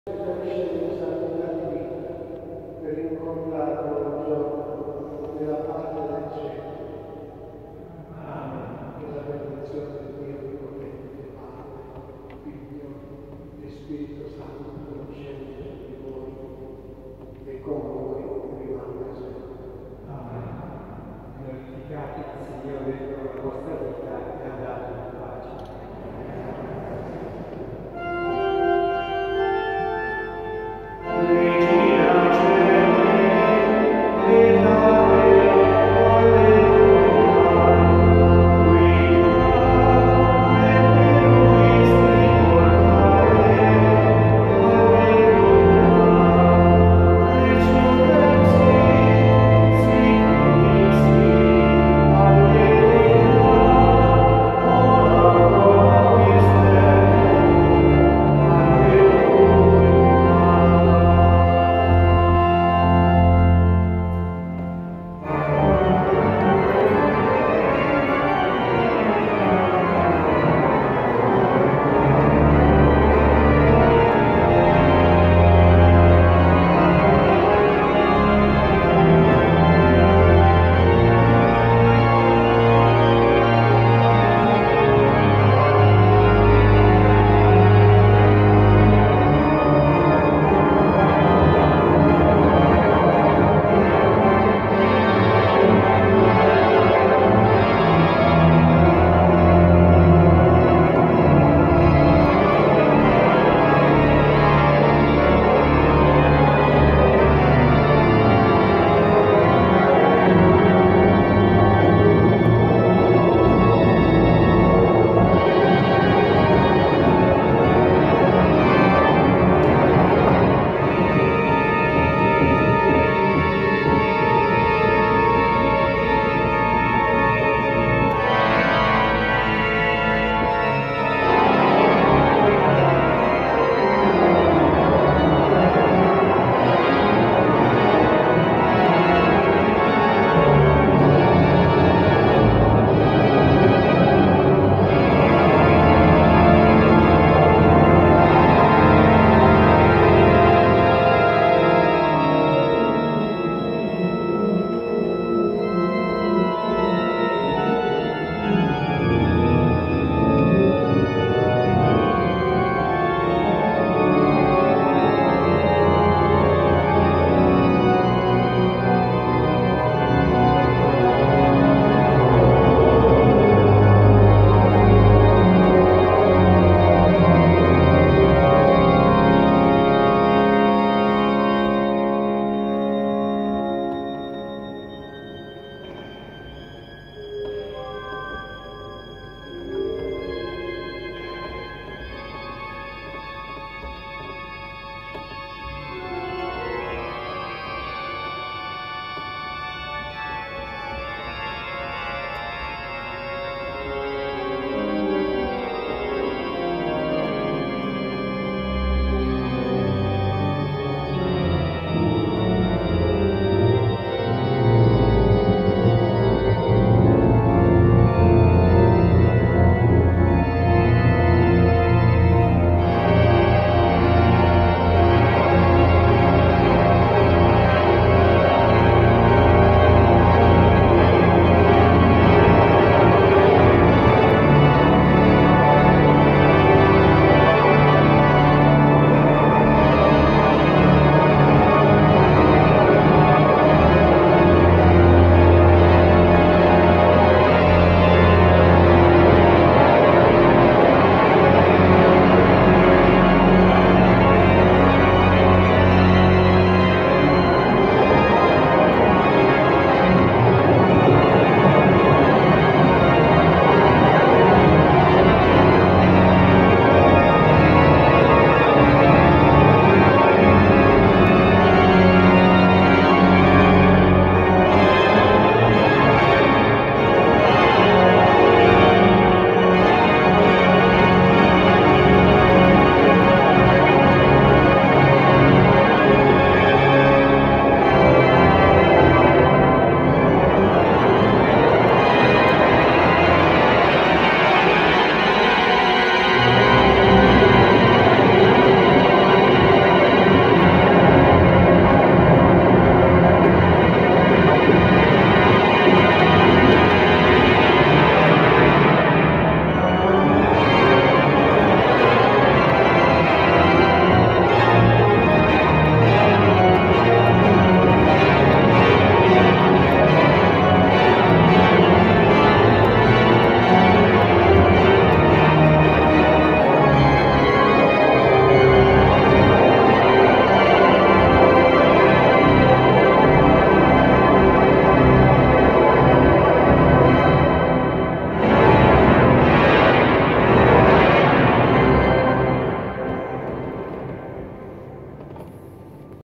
ORGAN IMPROVISATION 4 HANDS : RECESSIONAL 26-05-2019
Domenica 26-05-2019
VI di Pasqua
Congedobe Recessionale

Marco Agostinelli e Andrea Stacchietti all'organo Zanin della Cattedrale di Jesi